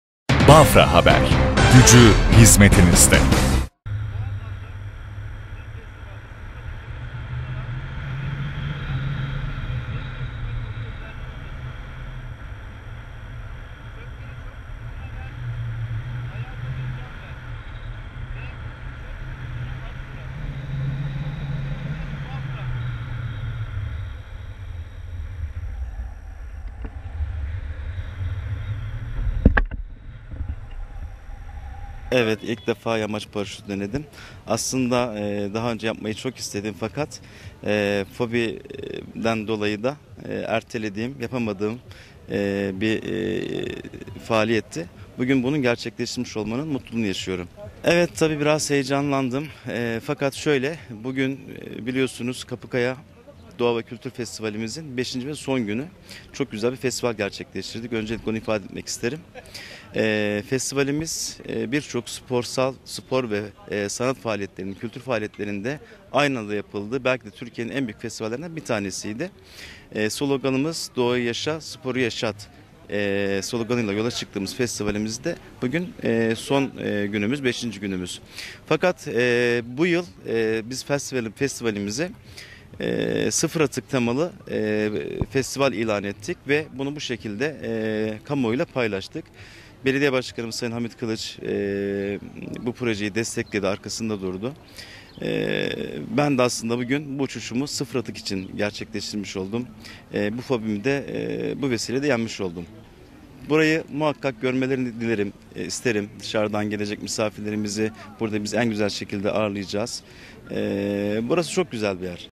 Yükseklik fobisini Sıfır Atık Projesi ile yendi
Samsun’un Bafra ilçesinde düzenlenen Kapıkaya Festivalinde ’Sıfır Atık Projesi’ için yükseklik korkusunu yendi.